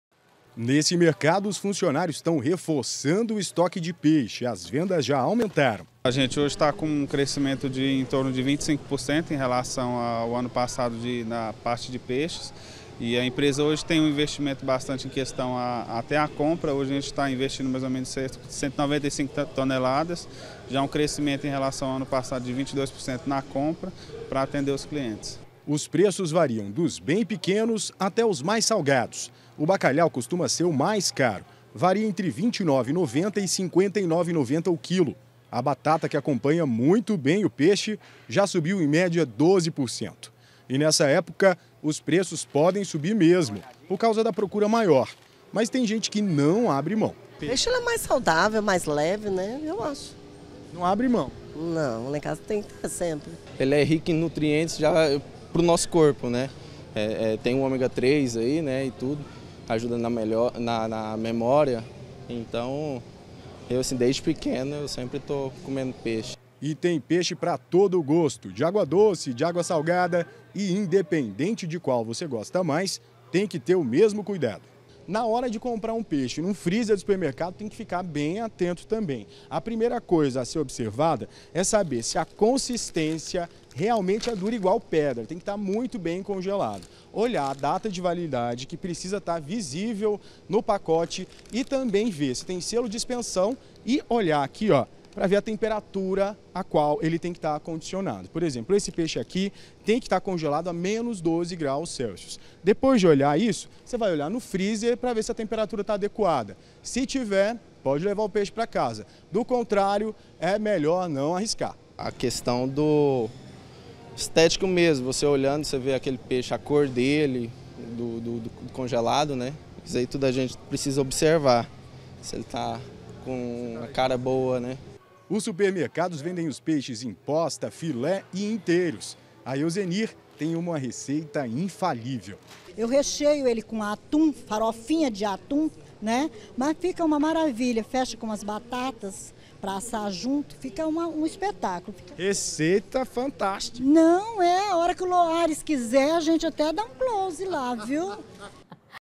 ÉPOCA DO ANO: PREÇO DO PEIXE ESTÁ MAIS CARO
O preço do peixe está mais alto. No supermercado o bacalhau pode ser encontrado de 29,90 a 59,90 o quilo. O segredo para comer peixe, gastando pouco, é pechinchar.